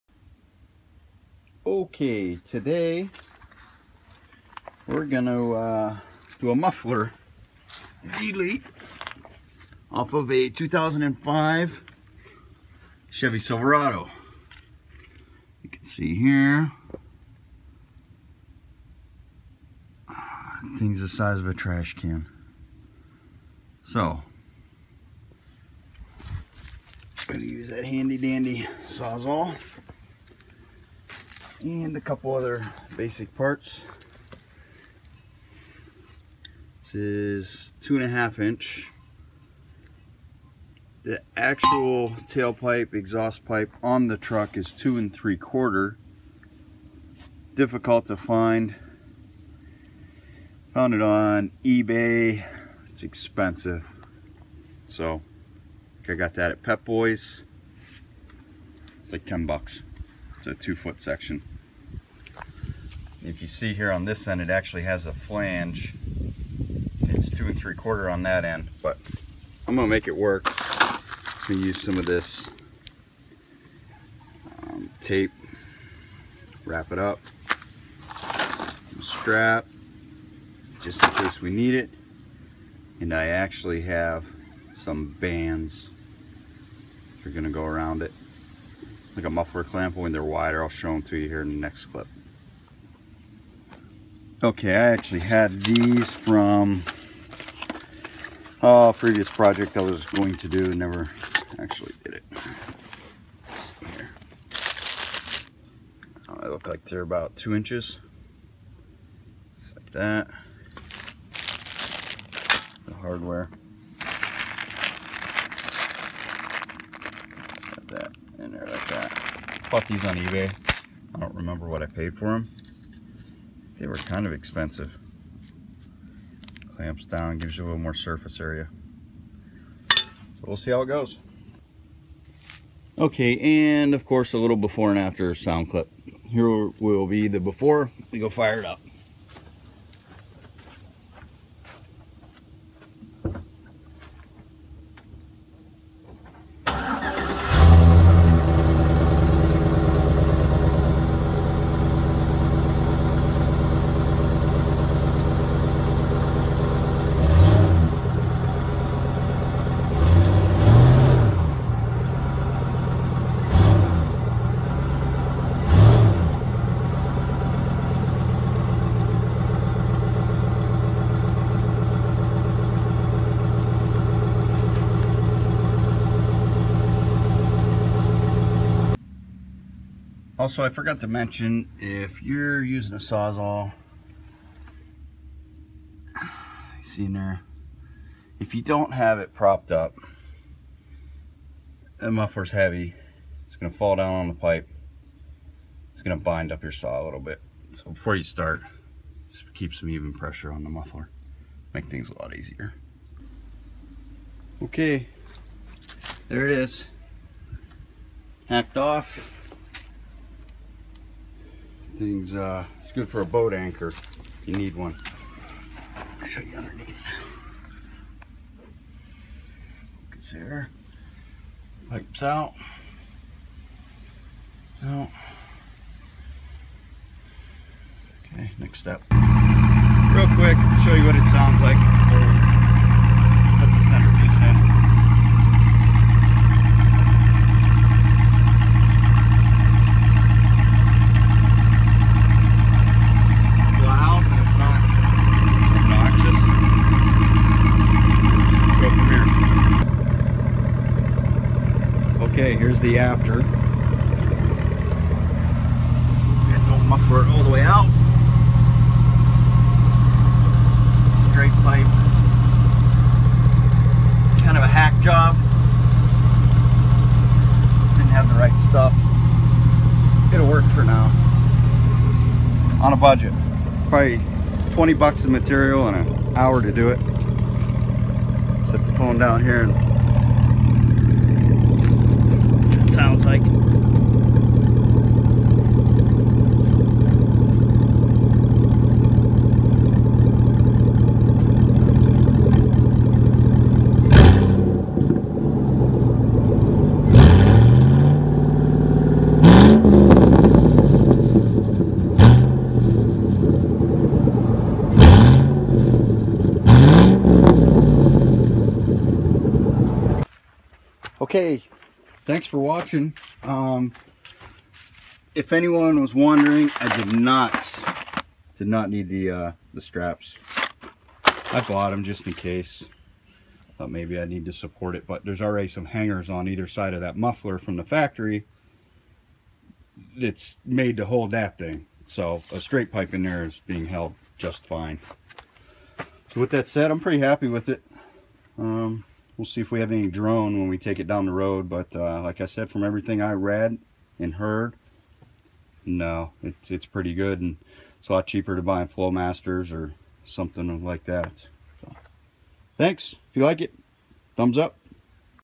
2005 Chevy Silverado 5.3 muffler delete
A quick, cheap, and easy way to wake up your truck! With a few basic tools you can remove your factory muffler and let your truck breathe.